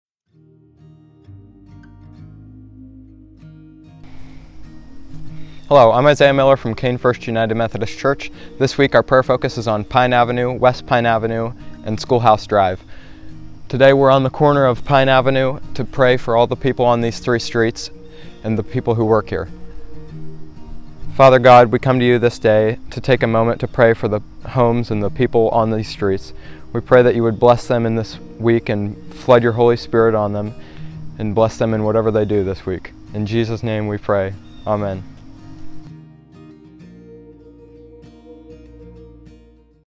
Schoolhouse Drive, West Pine Avenue, & Pine Avenue
At Kane First United Methodist for 2022 we are encouraging a renewed emphasis on prayer and spiritual renewal. Through the nudging of the Holy Spirit, we are beginning a street-by-street prayer saturation in the Kane Community.  It will be our desire to pray for a different street and all the residents, homes, and businesses on the designated street for that week.